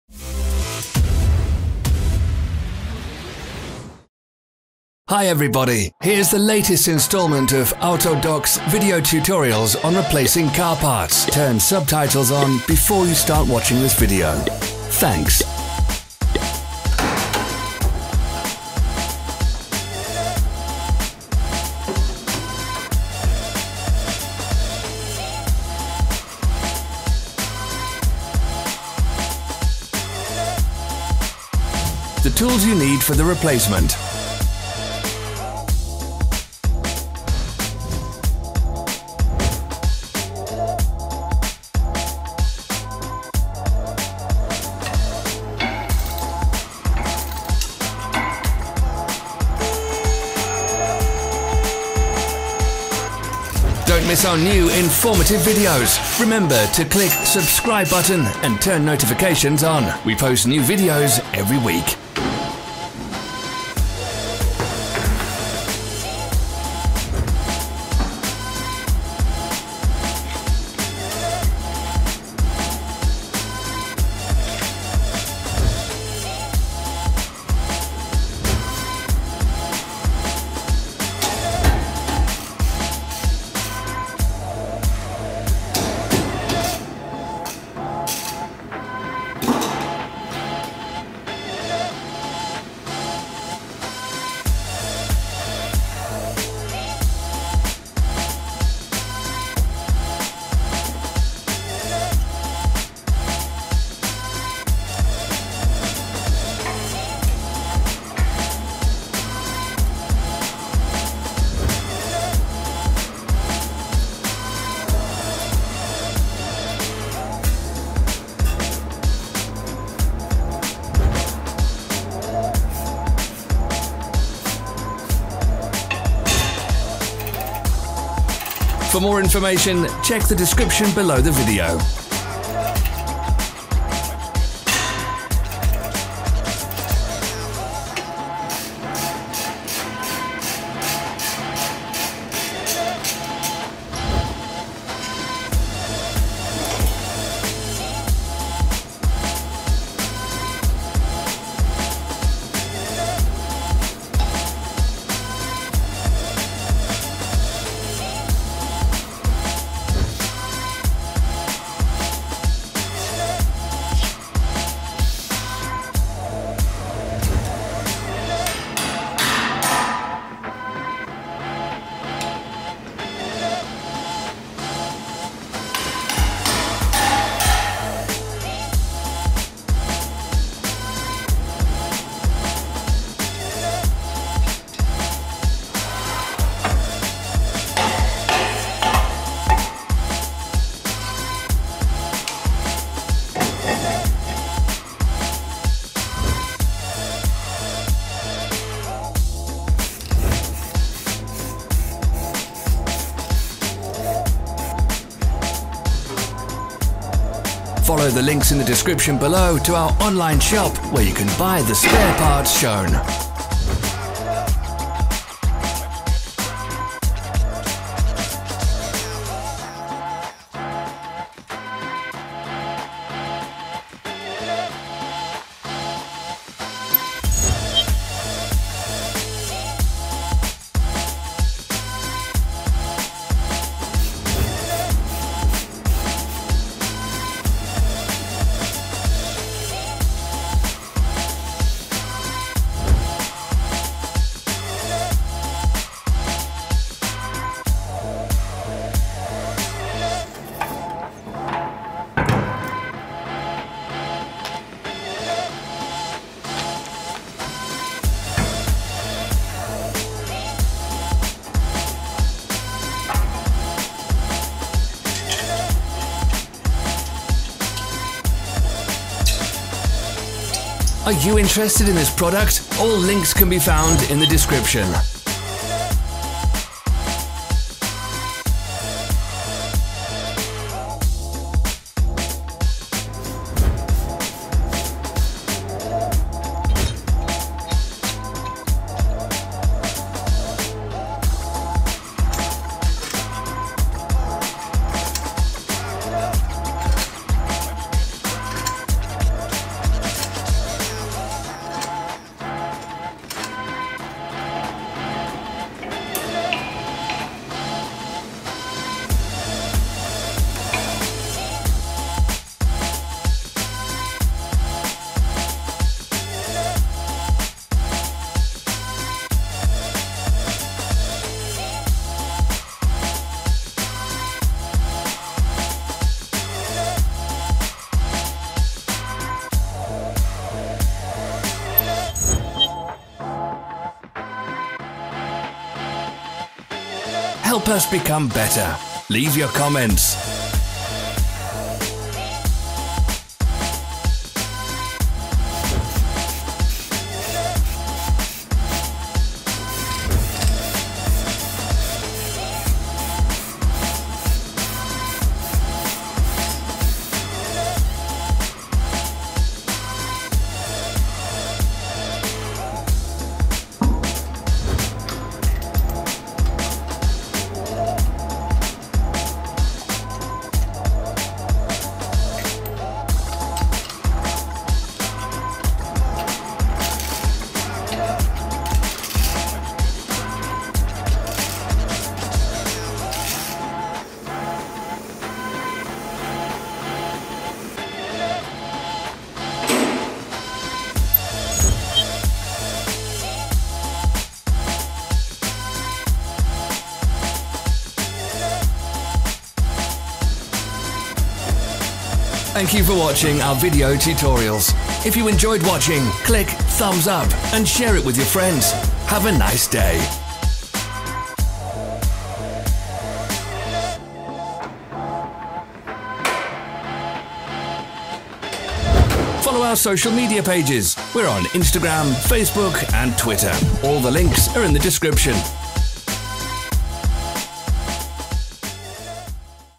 How to change front brake discs / front brake rotors on HONDA ACCORD 8 (CU2) [TUTORIAL AUTODOC]
How to change front brake discs / front brake rotors on HONDA ACCORD 8 (CU2) 2.4 Saloon 06.2008–present [TUTORIAL AUTODOC]

Brake discs HONDA Civic VIII Saloon (FD, FA) / HONDA Jazz II Hatchback (GD) / HONDA Odyssey (RA) / HONDA Prelude V (BB) cars should be replaced, as a rule, according to one and the same procedure. However, there can be slight differences due to specific features of the vehicle design.

🔝 Top brands that produce brake discs: RIDEX; ZIMMERMANN; STARK; BREMBO; BOSCH.

🛠️The tools you need to replace front brake discs:
- wire brush
- WD-40 spray
- brake cleaner
- copper grease
- wheel impact socket №19
- brake caliper wind back tool
- ratchet wrench
- torque wrench
- tap wrench
- hammer
- phillips screwdriver
- impact screwdriver
- rubber mallet
- crow bar
- wheel chock